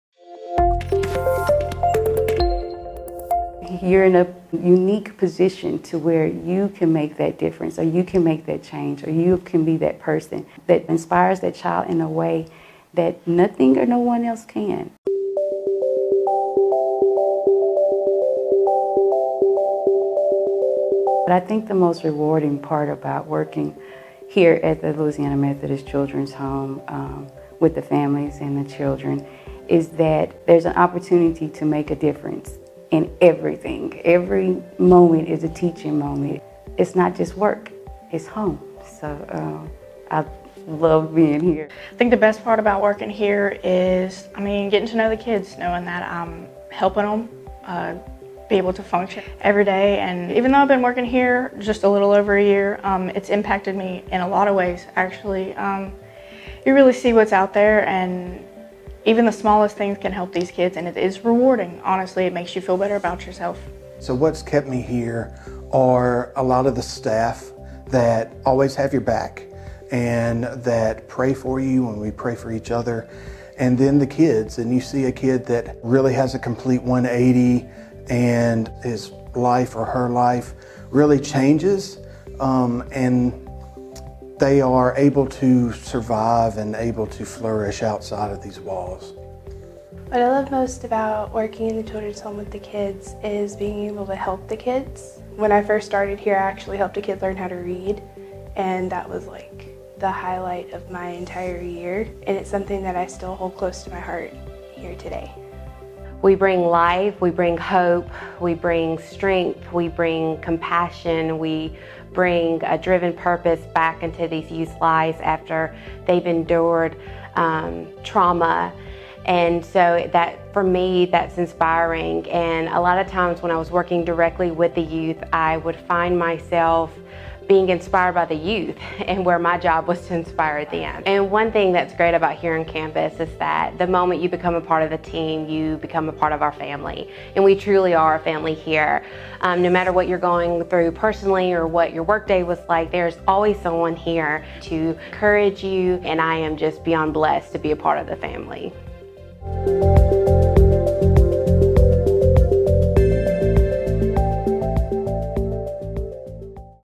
Join The MCH Family / Ministry on Mission
Meet more of our wonderful staff at The Louisiana Methodist Children's Home! Our mission is to provide hope and healing to children and families who need it most desperately. But don't take it from us, let our staff tell you what it's like working at the Children's Home!

Love what you see? We're hiring! to become a difference-maker!